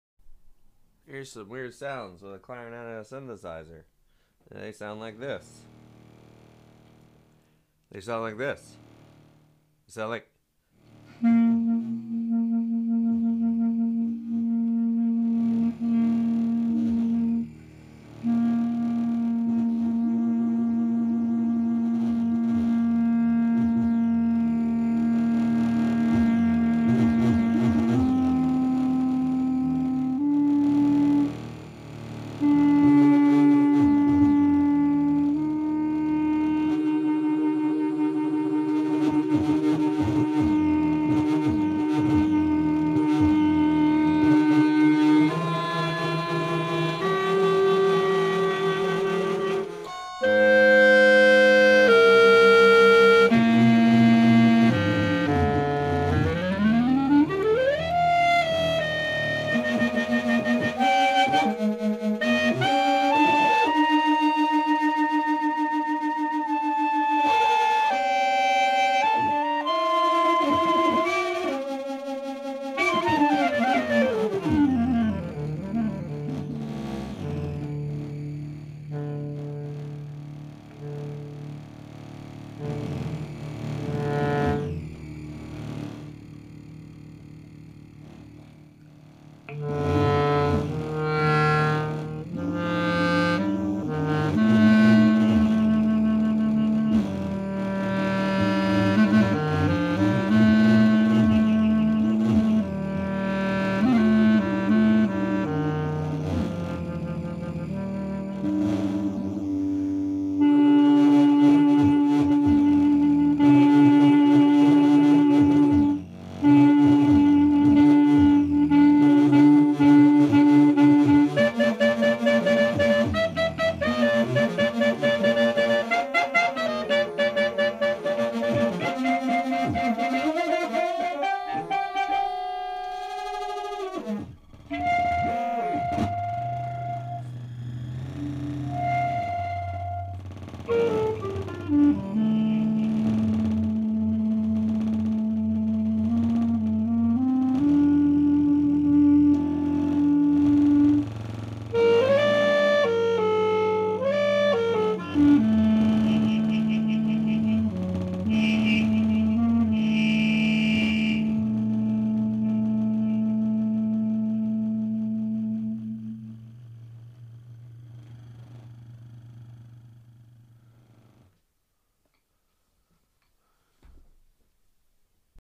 CV Clarinet using Behringer PP1
Adam Murphy, Milwaukee Wisconsin uses behringer perfect pitch pp1 module to convert clarinet audio from a piezo barrel pickup into CV control voltages for pitch, gate, and further audio manipulation using behringers 110, 140, and 150 modules, cr8audios chips module, and a diode chaos for woggle. Enjoy!